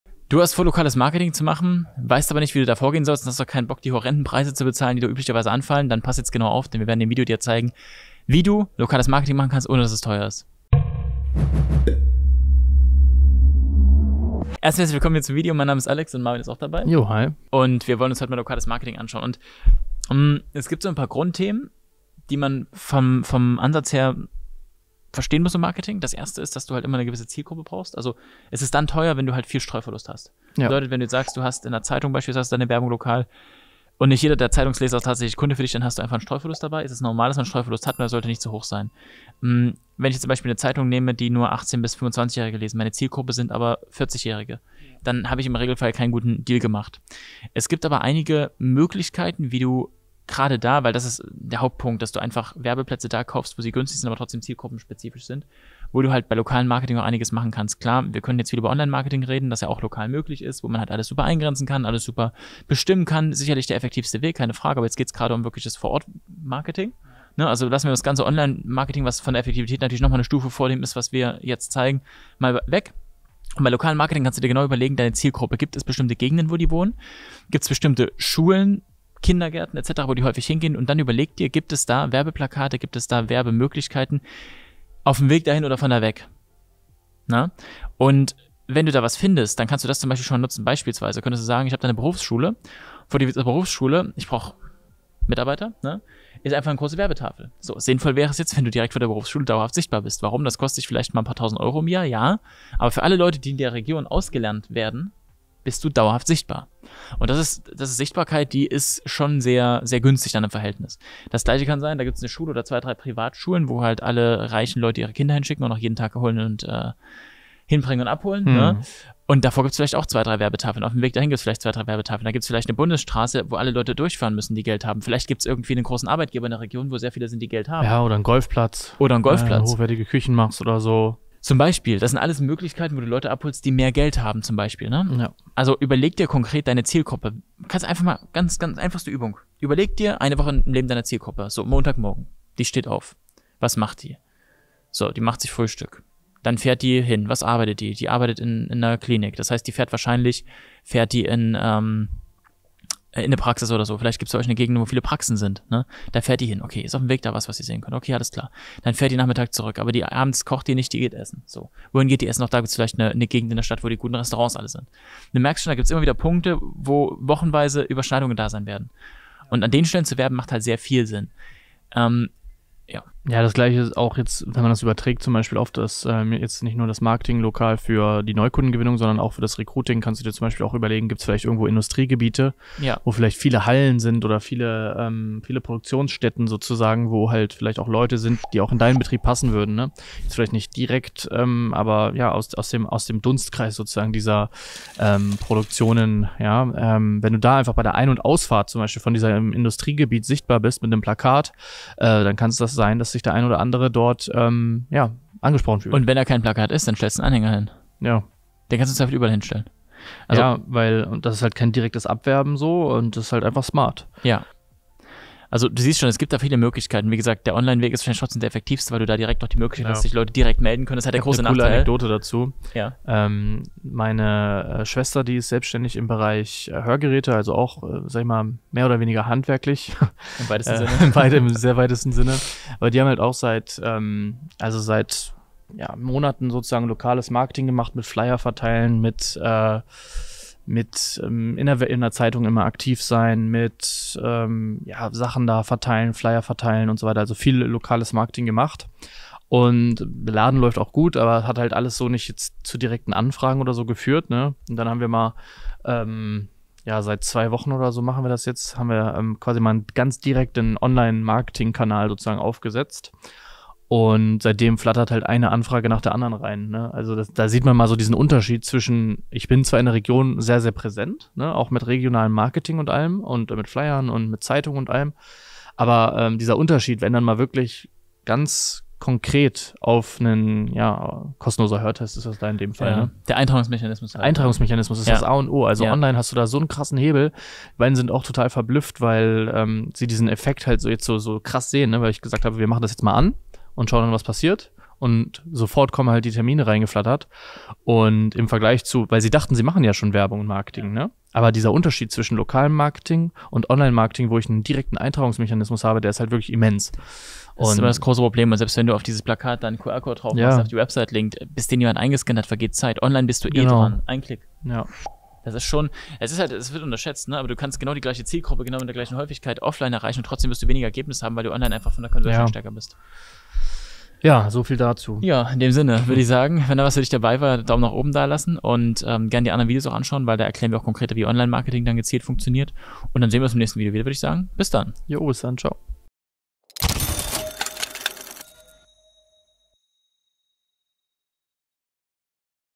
Lokales Marketing! Finde Kunden aus deiner Umgebung!
Lokales Marketing!? Wo liegen die Vorteile? Wie schafft man es, lokal sichtbar zu sein, um auch Kunden in der direkten Umgebung anzusprechen?
Online-Marketing bietet natürlich eine deutlich bessere und auch günstigere Möglichkeit, Kunden für sein Unternehmen zu gewinnen, aber auch das lokale Marketing hat durchaus Vorteile. Welche das sind? Das erfährst du in diesem Video.